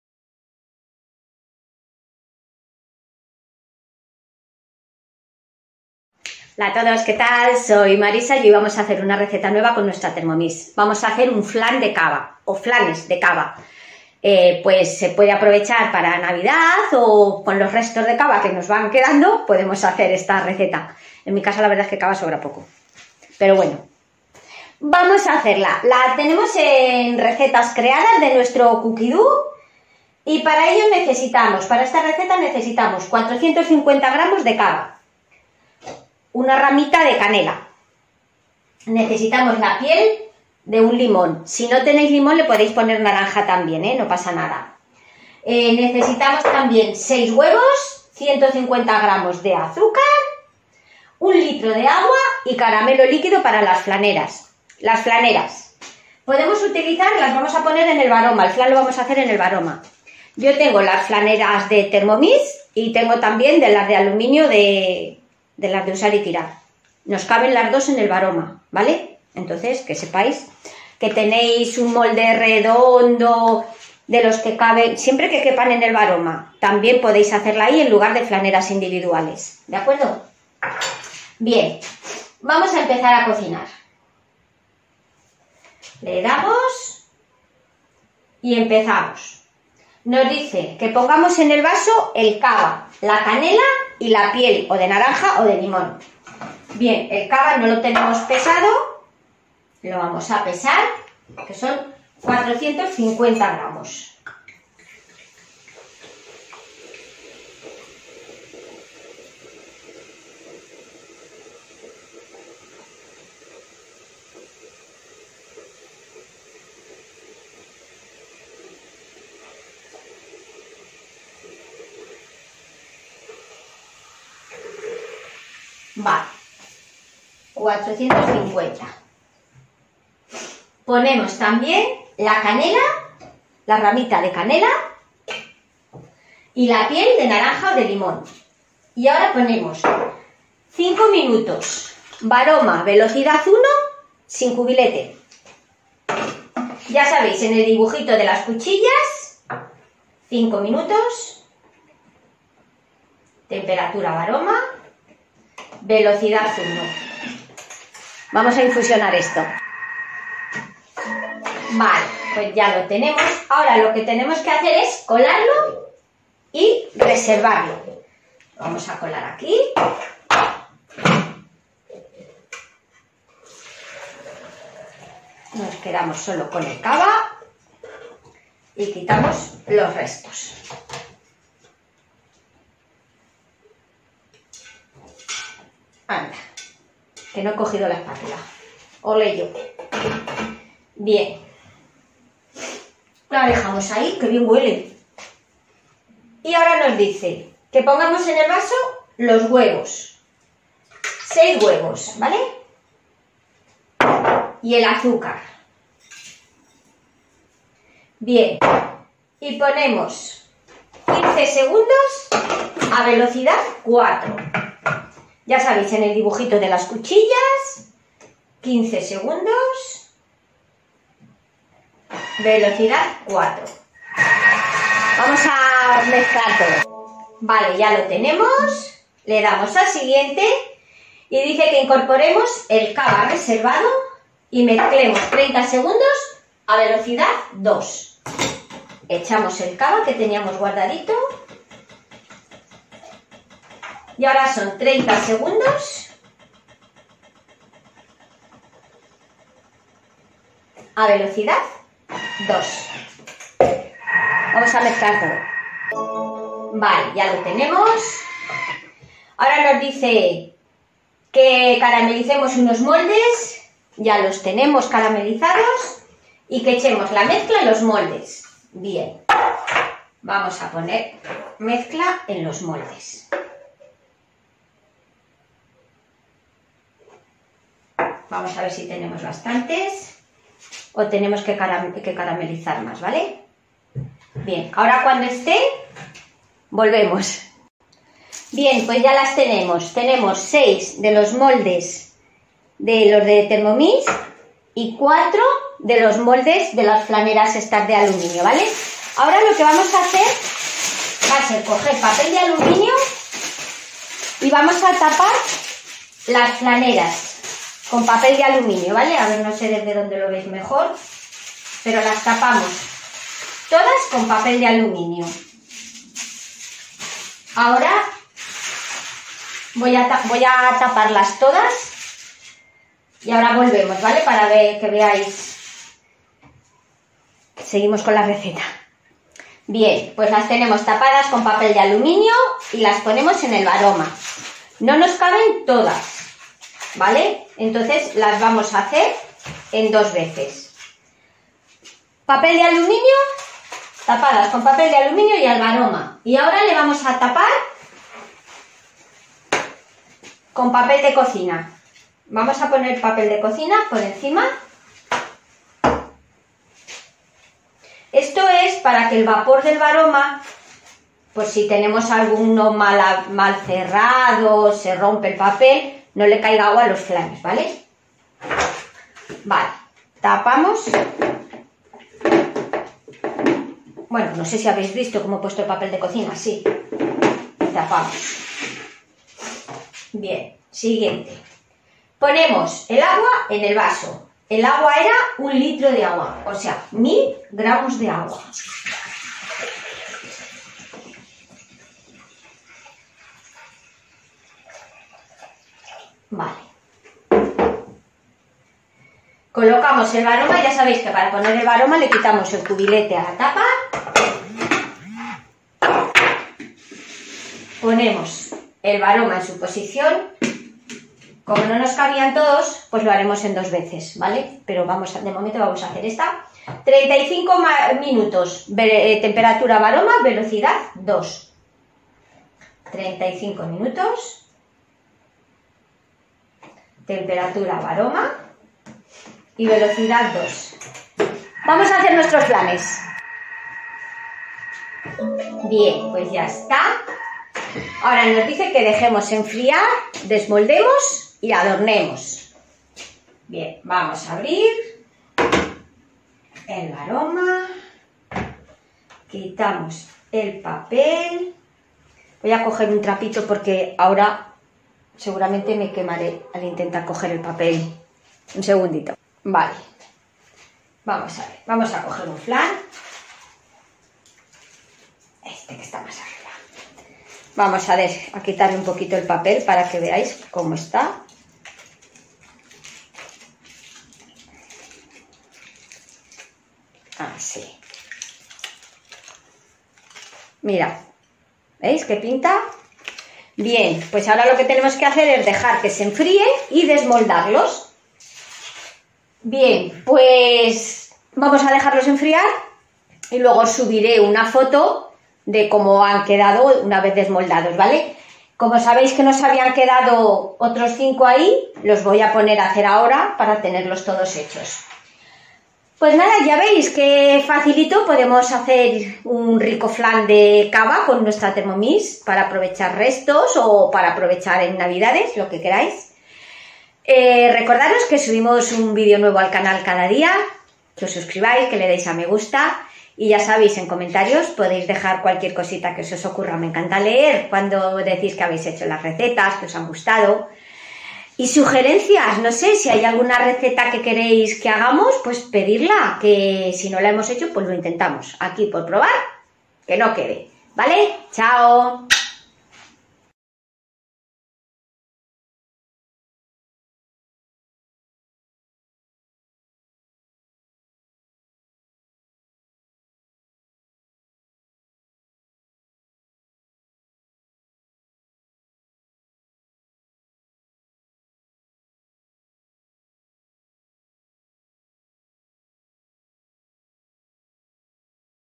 Flan de cava, fácil y rico con Thermomix
Ingredientes::450 g cava,1 rama de canela,la piel de un limón o naranja,6 huevos,150 g azúcar,1000 g de agua,caramelo líquido
Para decorar:fresas, caramelo líquido..
PREPARACIÓN
Ponemos en el vaso el cava,la canela y la piel de naranja o limón 5 min/varoma/vel 1 sin cubilete 10 min/70/ vel 1. Colamos y reservamos
Ponemos en el vaso los huevos y el azúcar 15 seg/ vel 4.Incorporamos el cava reservado y mezclamos 30 seg/ vel 2. Caramelizar unos moldes. Volcar la mezcla en los moldes. Cubrimos las flaneras con aluminio y ponemos en el varoma. Tapamos las flaneras con papel de aluminio y ponemos en el varoma. Tapamos con papel de cocina y tapamos el varoma
Ponemos el agua en el vaso,colocamos el varoma en su posición 35 min/varoma/vel 2.Enfriar y desmoldar. Adornar